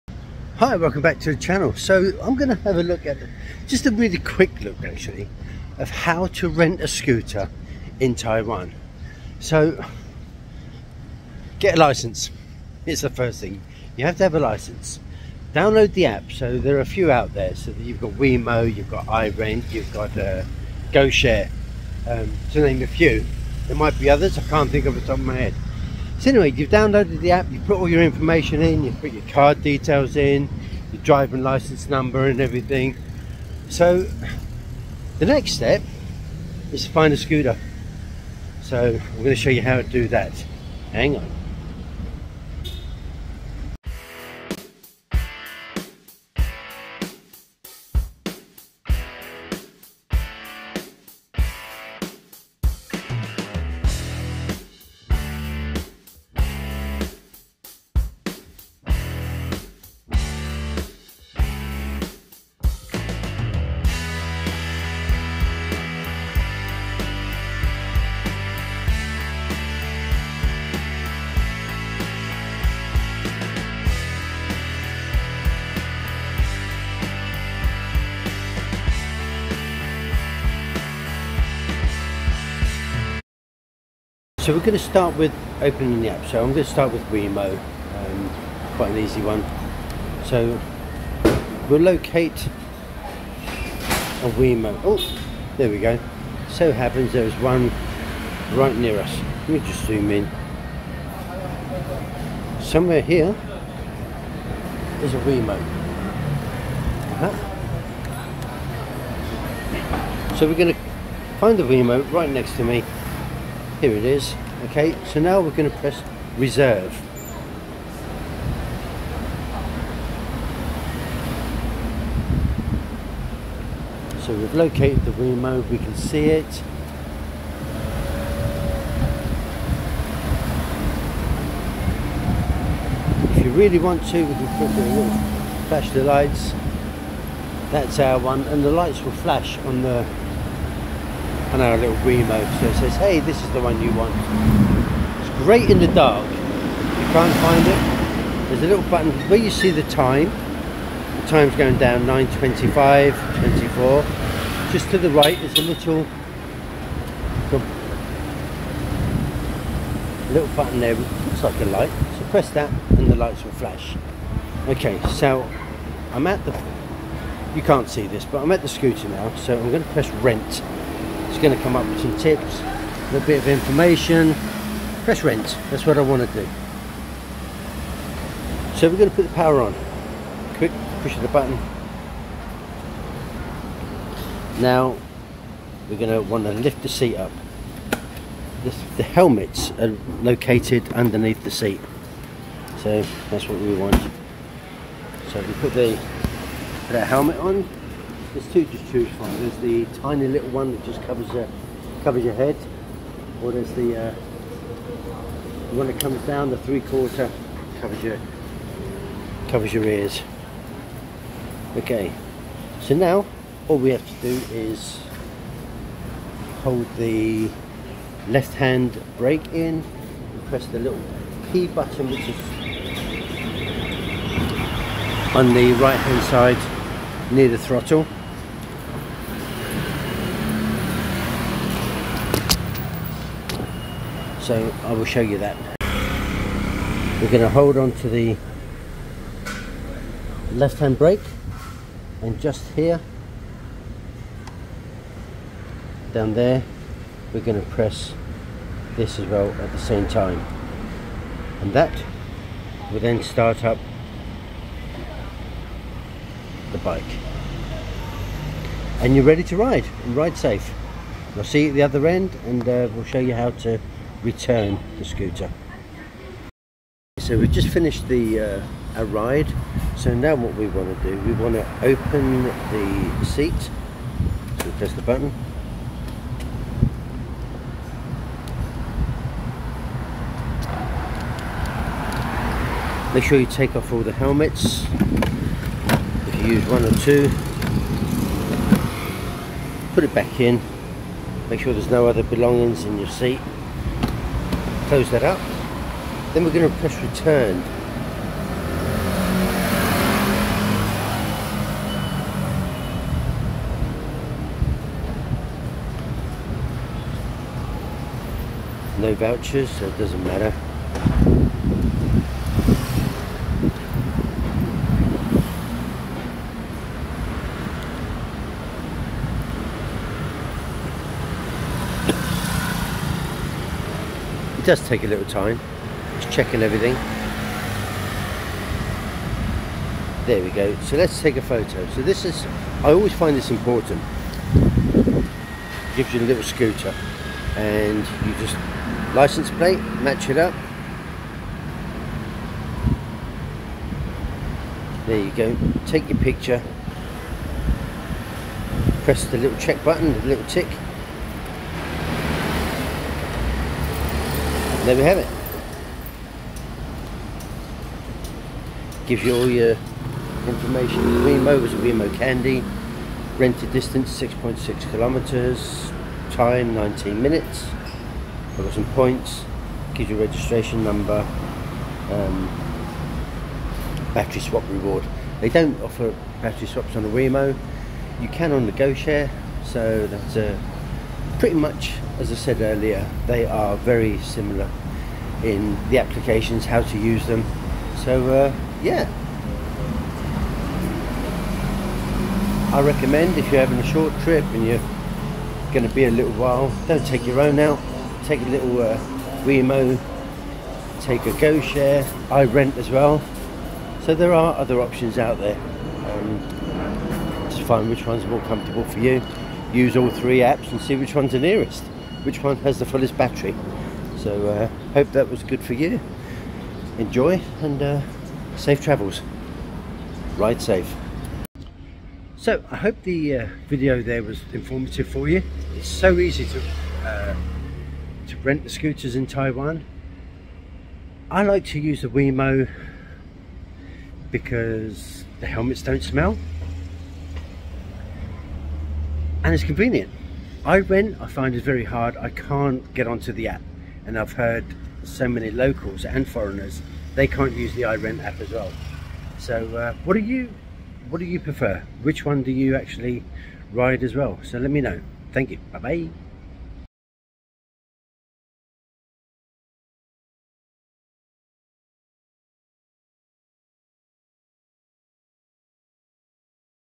Renting a scooter in Taiwan is as easy as 1, 2, 3. goshare irent wemo
I have had a few people ask me to make a video showing them the steps to rent a scooter. In this video I show you how to use an app (most apps are pretty much the same when it comes to renting) to start renting and also what to do on the app once you have riden to your destination and want to end the app and pay.